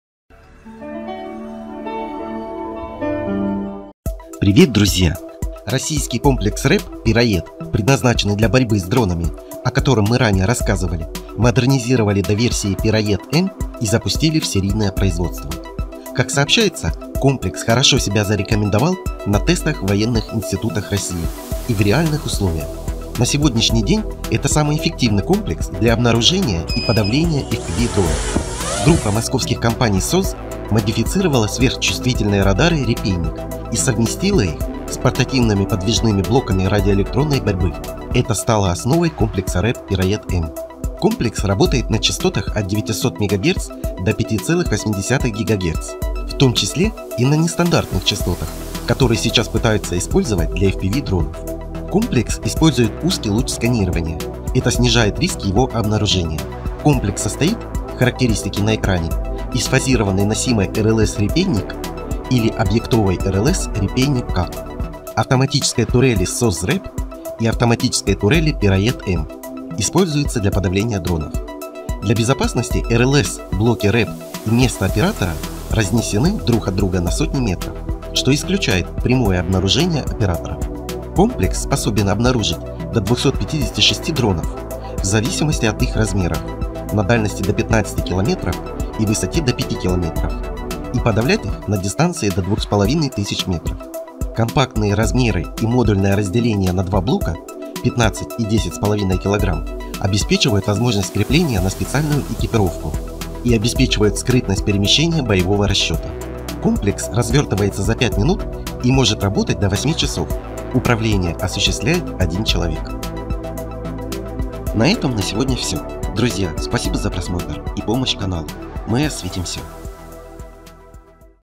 Выпуск РЭБ Пероед-М против дронов начат в России
Российский комплекс РЭБ «Пероед», предназначенный для борьбы с дронами, о котором мы ранее рассказывали, модернизировали до версии «Пероед-М» и запустили в серийное производство. Как сообщается, комплекс хорошо себя зарекомендовал на тестах в военных институтах России и в реальных условиях. На сегодняшний день это самый эффективный комплекс для обнаружения и подавления FPV-дронов. Группа московских компаний «СОЗ» модифицировала сверхчувствительные радары «Репейник» и совместила их с портативными подвижными блоками радиоэлектронной борьбы, это стало основой комплекса РЭБ «Пероед-М». Комплекс работает на частотах от 900 МГц до 5,8 Ггц, в том числе и на нестандартных частотах, которые сейчас пытаются использовать для FPV дронов. Комплекс использует узкий луч сканирования, это снижает риск его обнаружения. Комплекс состоит, характеристики на экране, из фазированной носимой РЛС «Репейник» или объектовой РЛС «Репейник-К», автоматической турели «СОЗ РЭБ» и автоматической турели «Пероед-М», используются для подавления дронов. Для безопасности, РЛС, блоки РЭБ и место работы оператора, разнесены друг от друга на сотни метров, что исключает прямое обнаружение операторов. Комплекс способен обнаружить до 256 дронов, в зависимости от их размеров, на дальности до 15 км и высоте до 5 км и подавлять их на дистанции до 2500 метров. Компактные размеры и модульное разделение на два блока 15 и 10,5 кг обеспечивают возможность крепления на специальную экипировку и обеспечивают скрытность перемещения боевого расчета. Комплекс развертывается за 5 минут и может работать до 8 часов, управление осуществляет один человек.